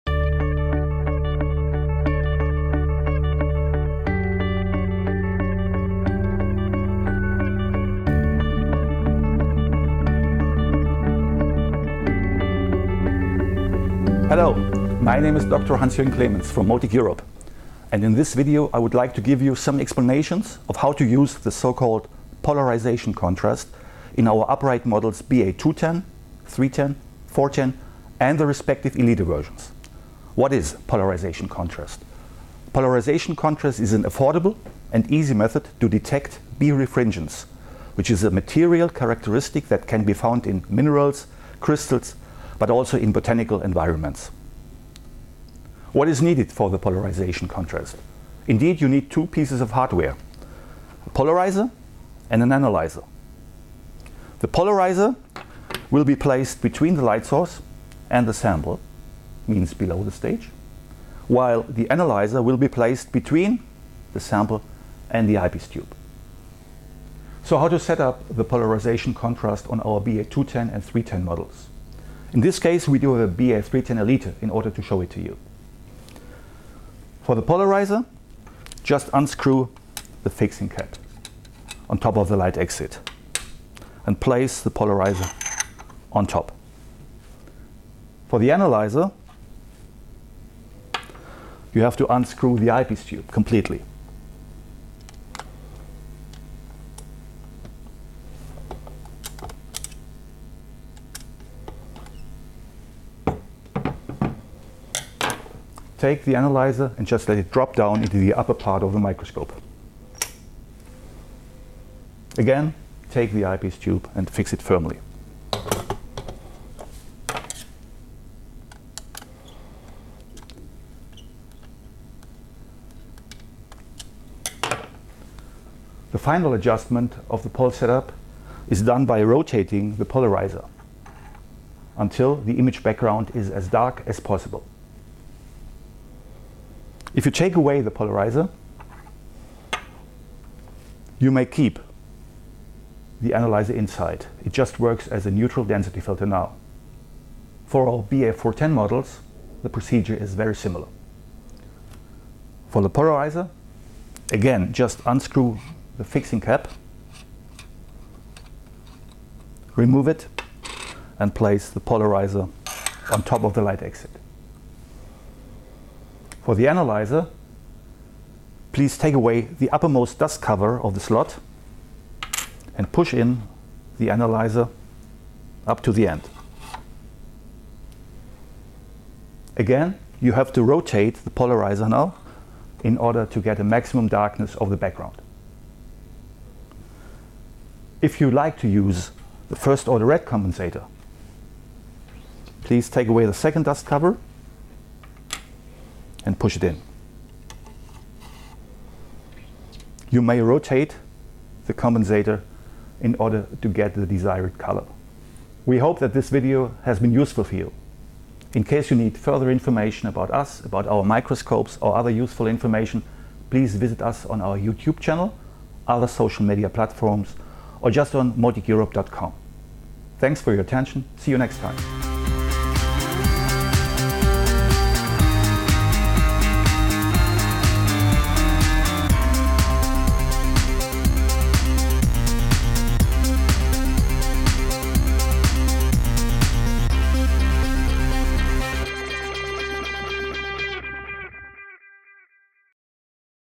Contrast Methods - Polarizing system | by Motic Europe
Discover how to use the polarization contrast accessories in our BA210, BA310 and BA410 upright microscopes, including the Elite versions. By our product specialist Dr. Hans-Jürgen Klemenz.

✔️ Subtitles available in Spanish, German, French and Italian.